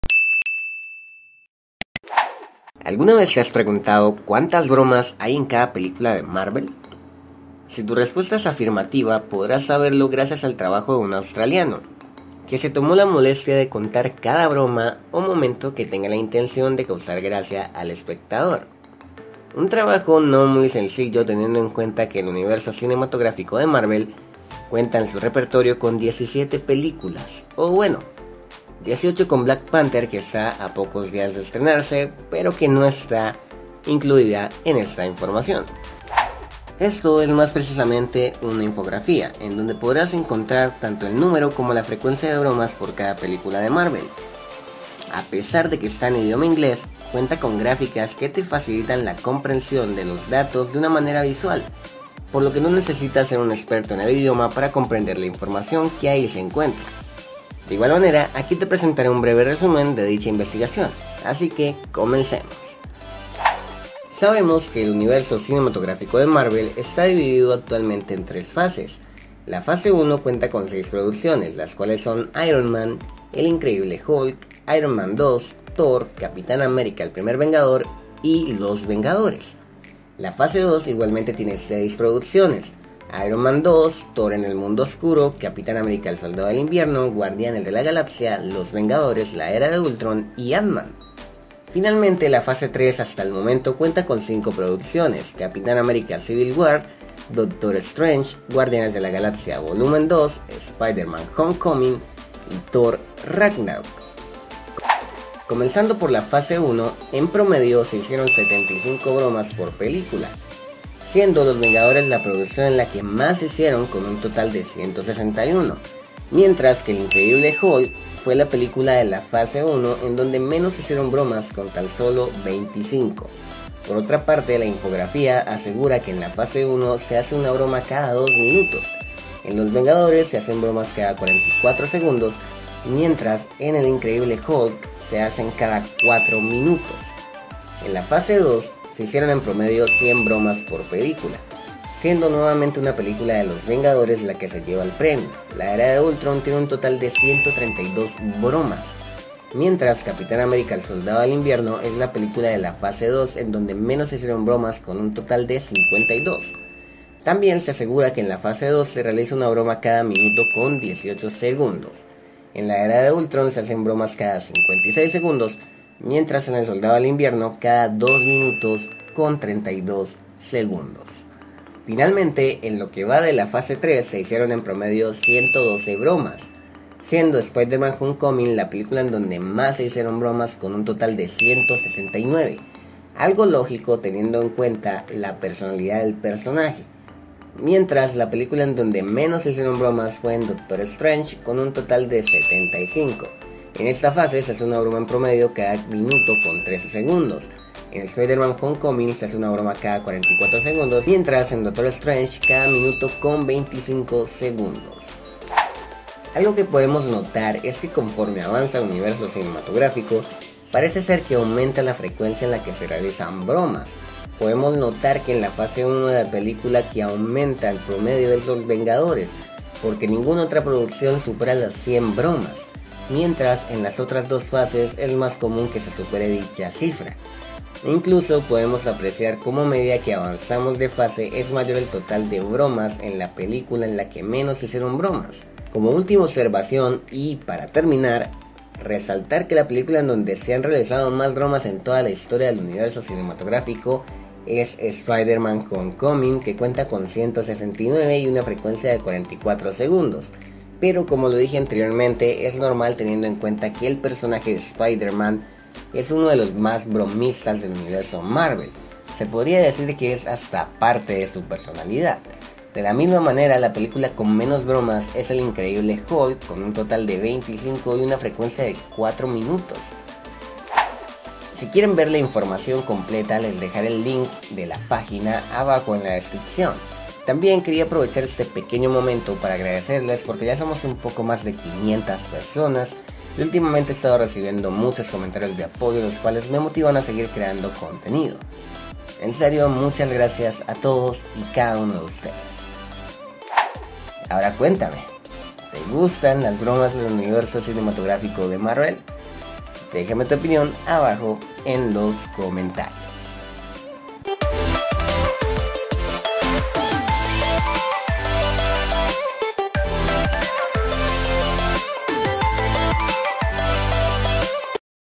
¿Cuantas Bromas Se Hacen En Cada Película De Marvel?
¿Alguna vez te has preguntado cuantas bromas hay en cada película de Marvel? Si tu respuesta es afirmativa podrás saberlo gracias al trabajo de un australiano, que se tomó la molestia de contar cada broma o momento que tenga la intención de causar gracia al espectador, un trabajo no muy sencillo teniendo en cuenta que el universo cinematográfico de Marvel cuenta en su repertorio con 17 películas, o bueno, 18 con Black Panther que está a pocos días de estrenarse.

Es una infografía en donde podrás encontrar tanto el numero como la frecuencia de bromas por cada película de Marvel, a pesar de que esta en idioma inglés, cuenta con graficas que te facilitan la comprensión de los datos de una manera visual, por lo que no necesitas ser un experto en el idioma para comprender la información que ahí se encuentra.